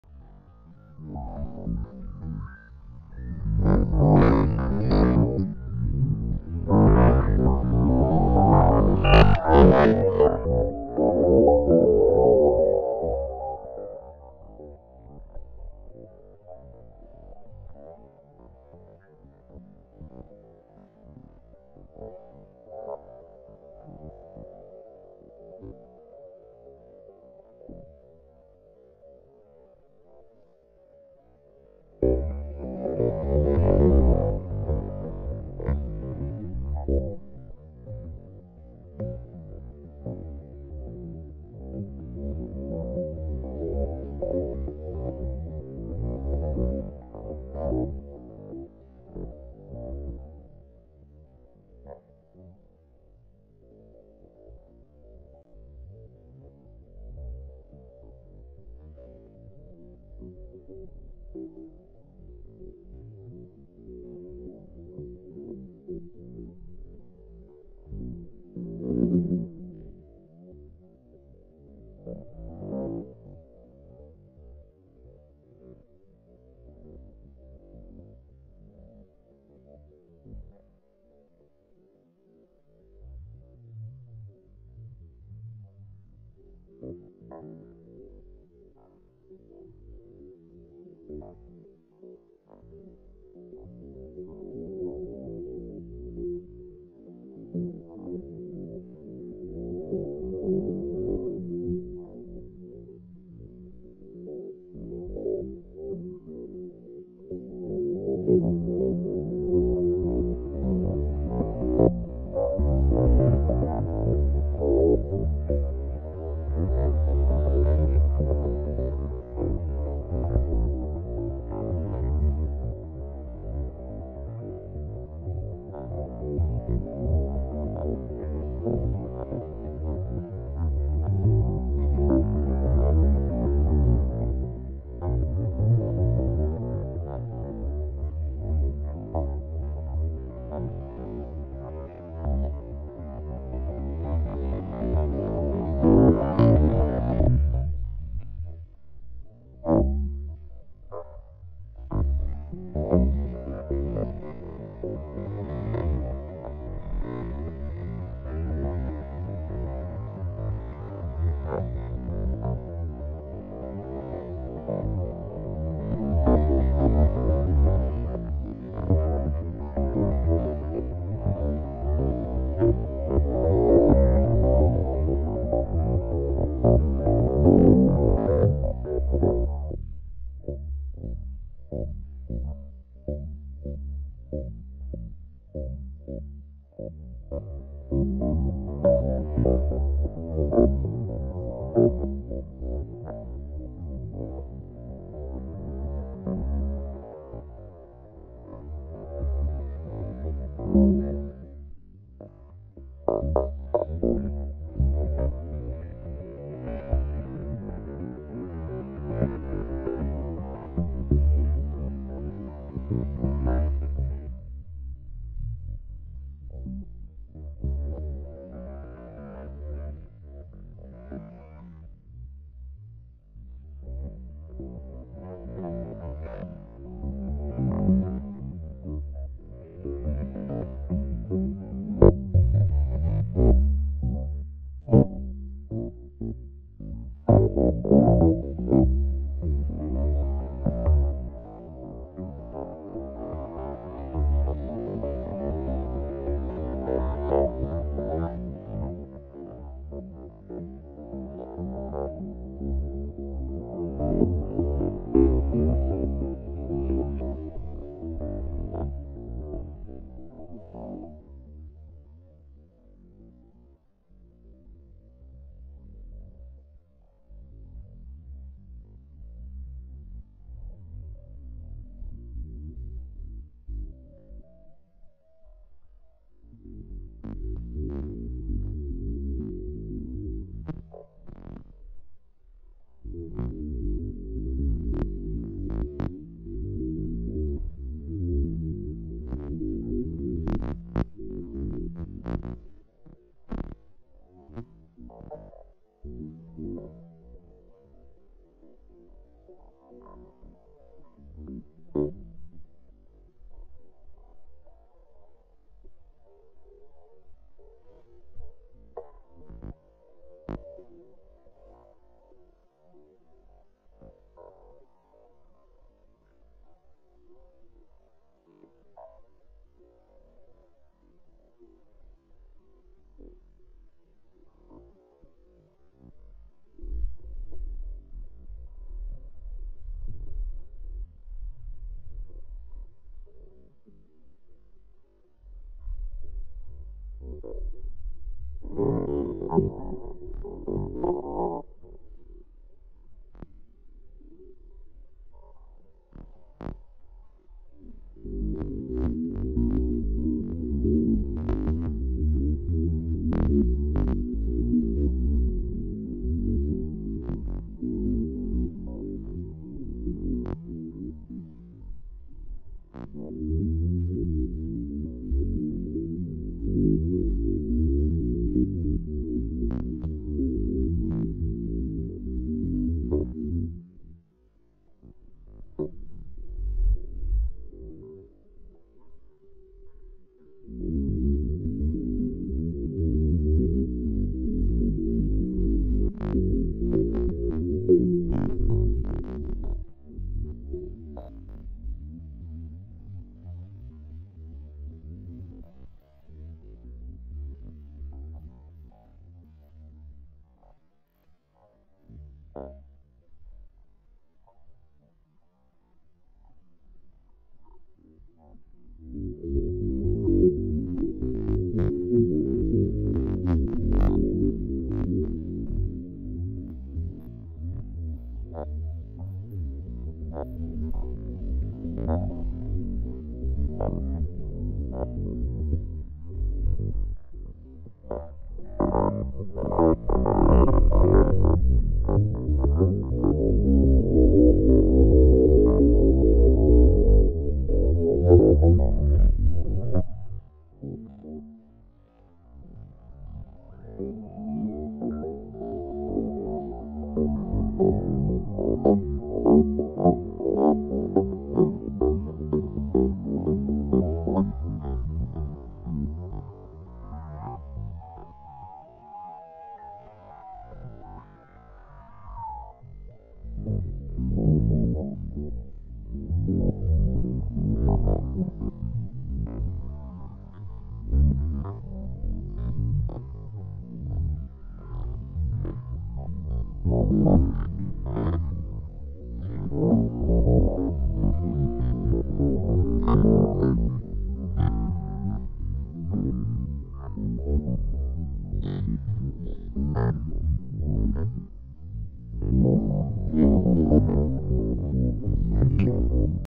Camera Fishing, Portmahomack
Casting a camera into the sea from a harbour wall, and seeing what we "catch", as in footage. There's plenty of fish!

The camera is a GoPro Hero8 on a custom rig to stabilise it a little and give slight negative buoyancy, and shot in wide angle HD at 120fps, then slowed down 4x

Audio up if you like psychedelic-found-jazz ;)